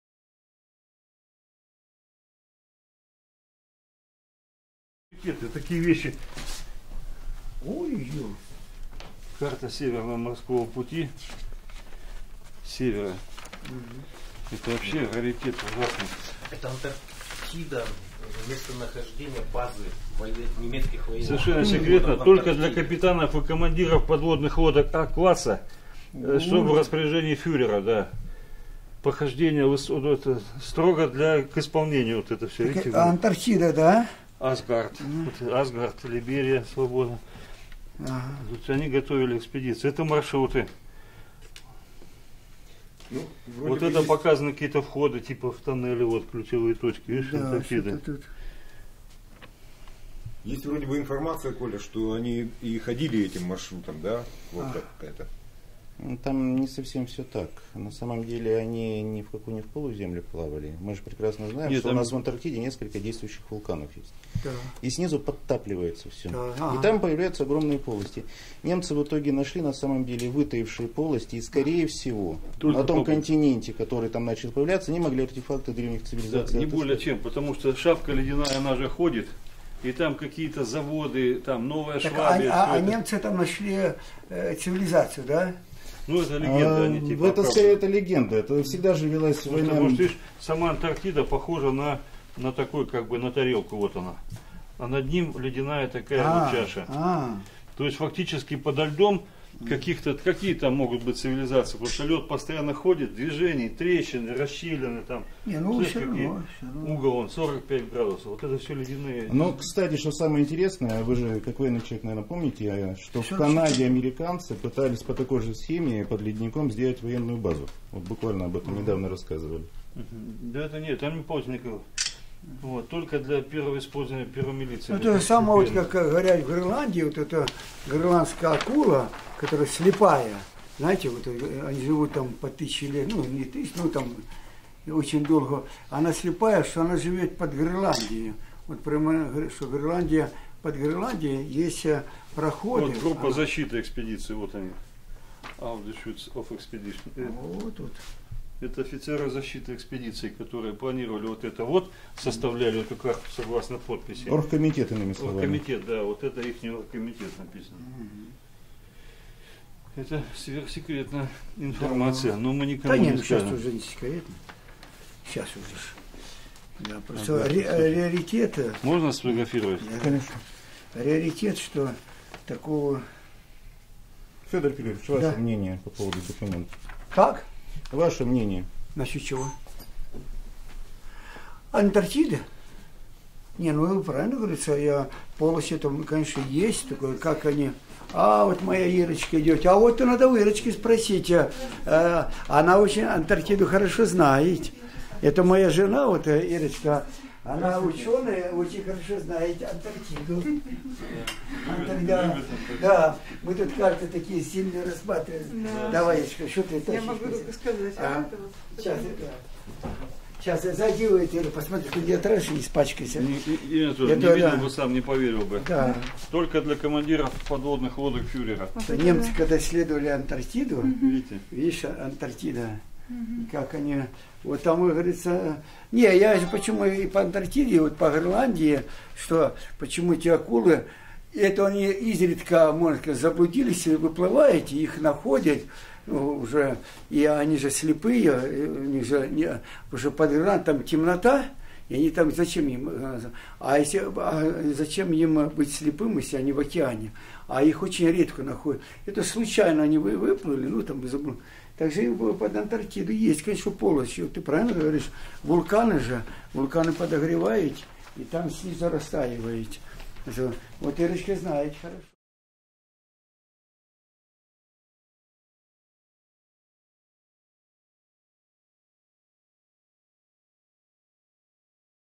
Карта полой Земли. Фёдор Конюхов, Николай Субботин
Показываю Фёдору Конюхову оригиналы материалов из моего архива, на основе которого Леонид Ивашов написал книгу "Опрокинутый мир". Оригиналы карты для немецких подводников и синяя тетрадь с записями конспектов методических рекомендаций для формирования антарктической команды для проекта "Новая Швабия".
✅ Наш подписной канал с полными версиями интервью и фильмов, которые никогда не выходили и не выйдут на других площадках